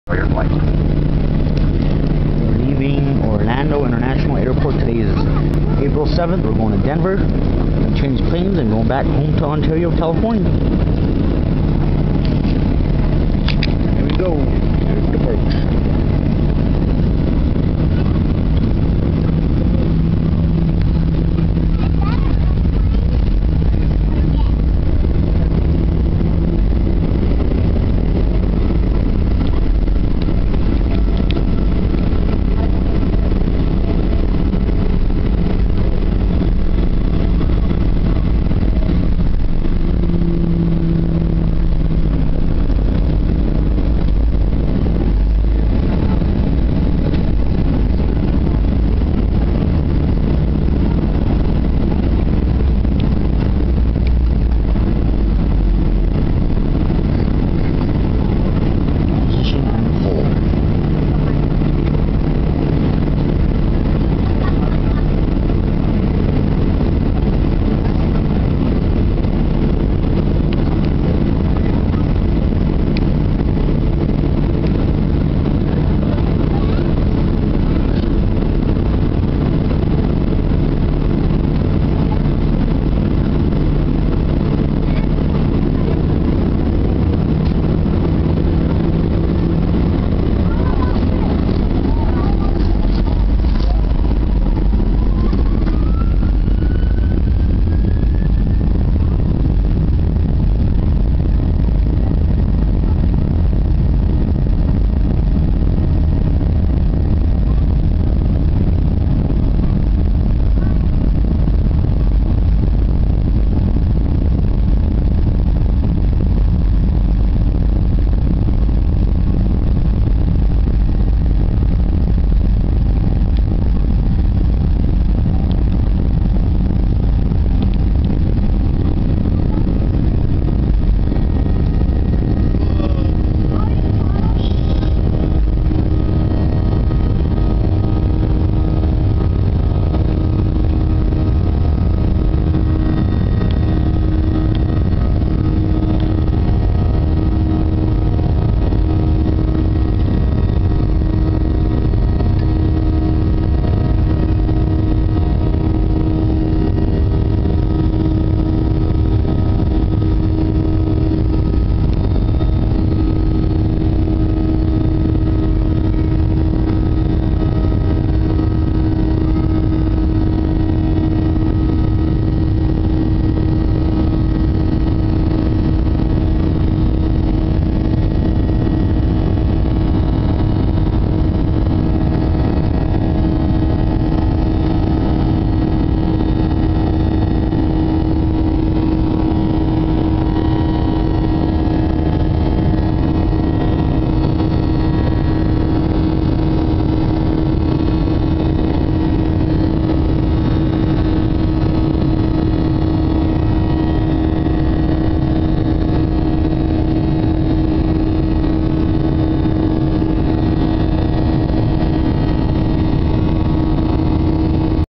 Southwest Airlines Take-Off
On 04-07-10, this is the taxi and take-off phase, departing from MCO (Orlando International Airport) of my initial flight home onboard the Southwest Airlines Boeing 737-700 jet. This flight stopped over at Denver International, and then I changed planes for my final journey home to Ontario, California. Florida is a very beautiful place!